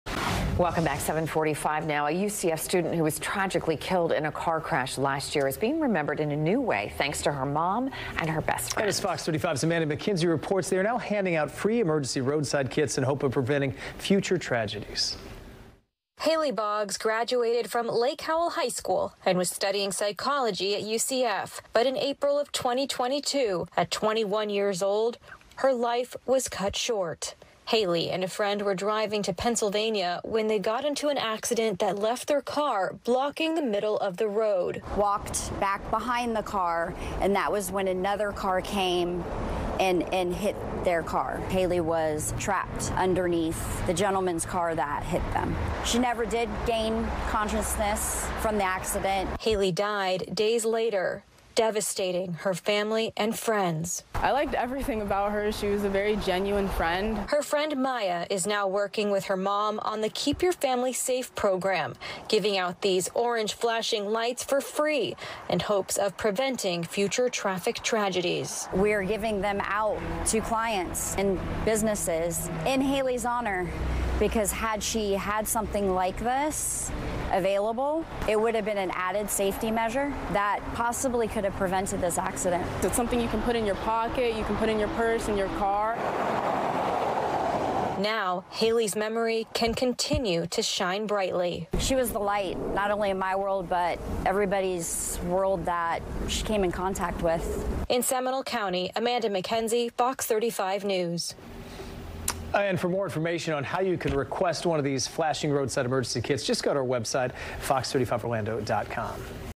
Family, friends of UCF student killed in crash hand out free emergency lights in her honor
A University of Central Florida student who was tragically killed in a car accident last year is being remembered in a new way thanks to her mom and best friend.